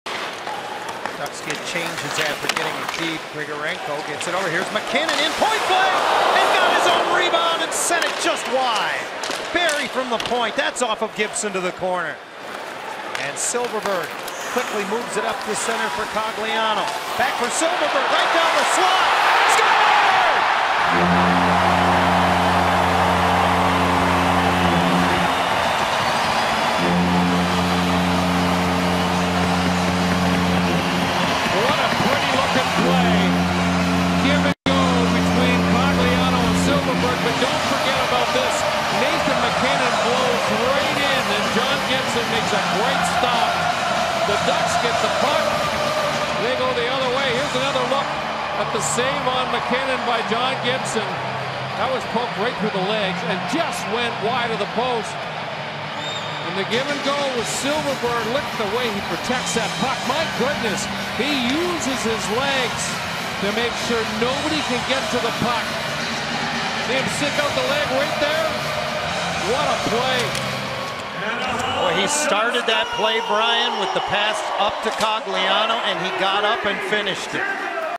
Silfverberg rewards his goalie's timely save with a magnificent goal
To thank his goalie John Gibson for a timely save, Anaheim Ducks forward Jakob Silfverberg scores a thrilling goal on the ensuing rush the other way.